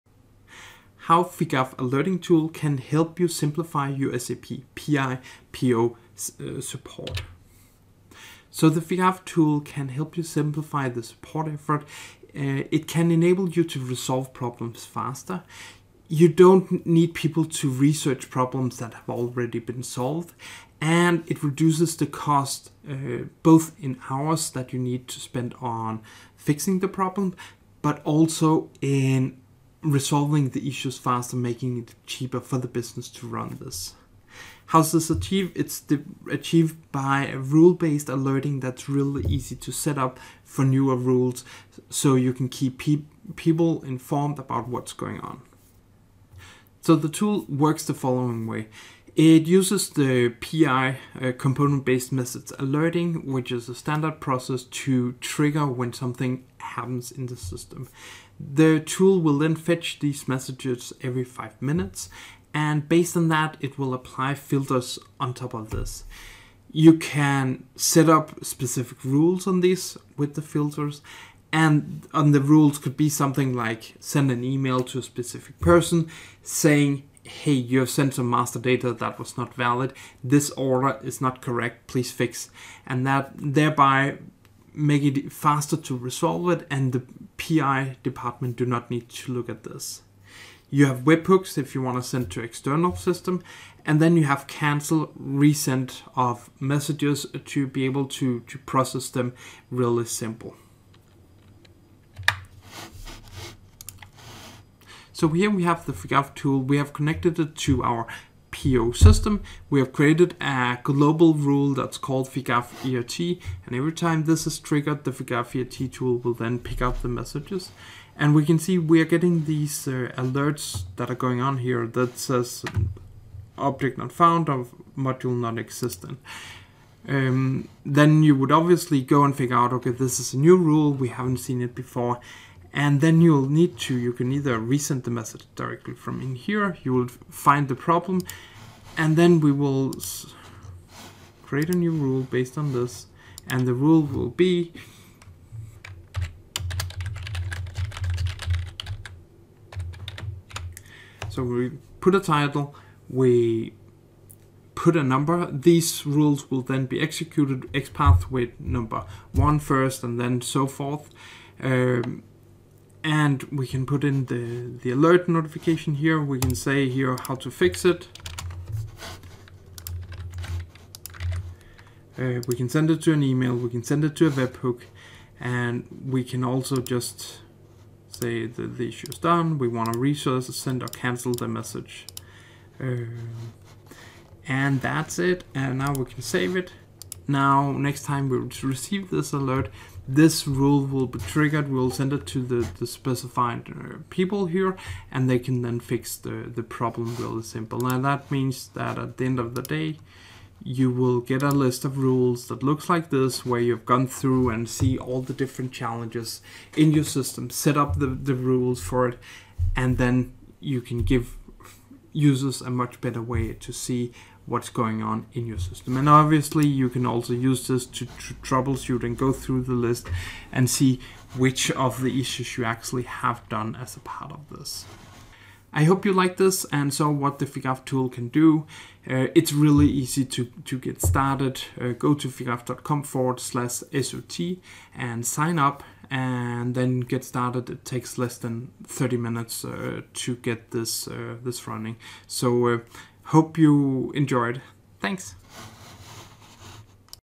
Figaf Alert Tool for SAP PI/PO
In this video you can see how you can automate your SAP PI/PO alerting.